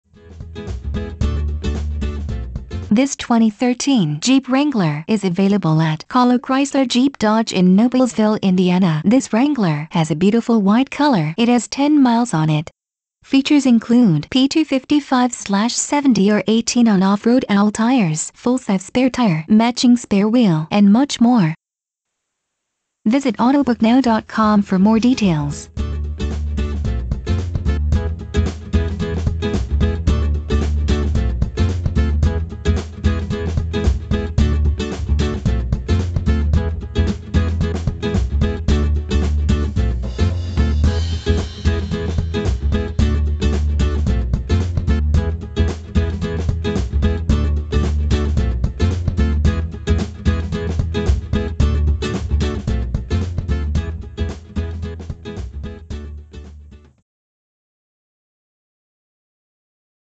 2013 Jeep Wrangler Unlimited in Noblesville IN for Sale
2013 Jeep Wrangler Unlimited
Stock #: 3J23
Price: $34,345
Miles: 10
Trim: Unlimited
Body Type: SUV
Color: White
Transmission: Manual